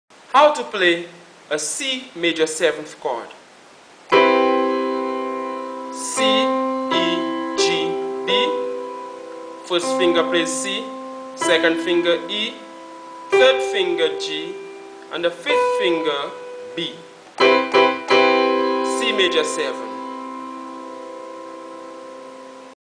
How To Play Cmaj7 Chord (C major seventh, C maj 7th) On Piano & Keyboard
Learn how to play the Cmaj7 chord in this short video. To learn how to play more chords, go How to play the C major seventh chord on keyboard and piano. C maj 7th. Notes of C major 7th chord are C, E, G and B. Fingering is 1 2 3 5.